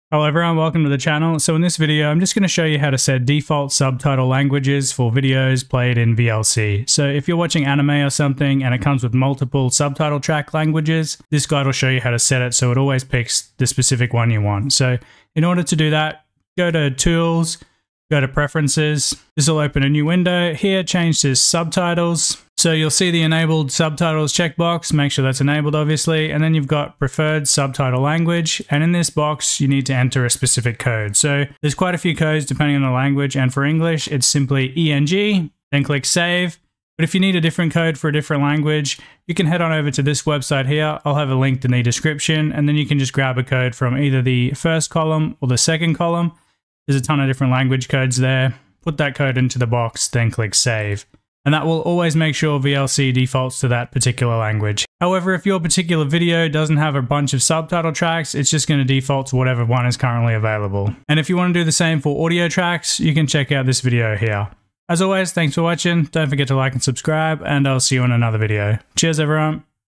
How to Set the Default Subtitle Language in VLC
If you're watching content with multiple subtitle tracks in VLC, such as foreign films or anime, this guide will show you how to set VLC to always use the same subtitle language by default. For example, if you often watch anime with English subtitles, you can configure VLC to always display English subtitles when available. You can also use this guide to configure your preferred audio track as well.